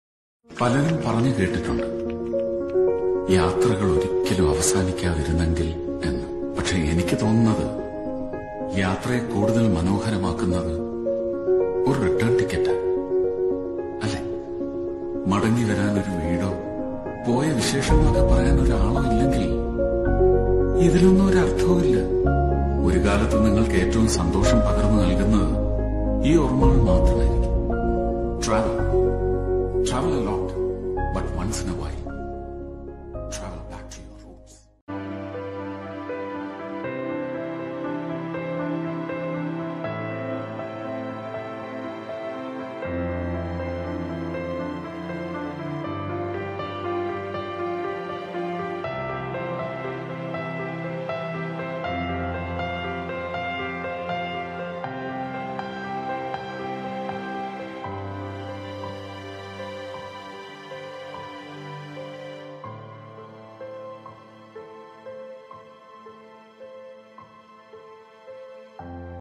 entekeralam trip kerala kozhikode keralatourism vayalada roadtrip mallu travel travelvlog
Vayalada viewpoint (Mullanpara) is around 15 kilometers from balussery junction and around 38 kilometers from calicut.you will get a panoramic view from the clifftop .an ideal place for trekking groups where you can traverse around the forest on the way to the viewpoint